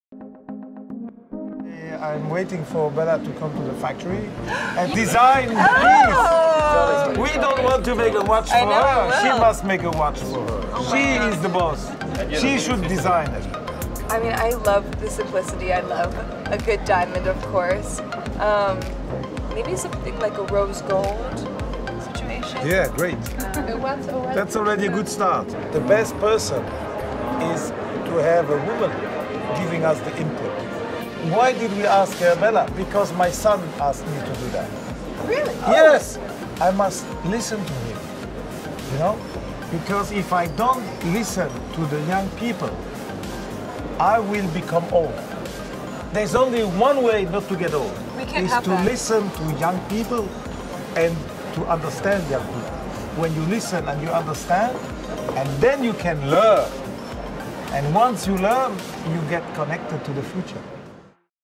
Bella Hadid becomes TAG Heuer Ambassador
To mark New York Fashion Week, TAG Heuer and Bella Hadid chose Equinox Bond Street to announce their collaboration.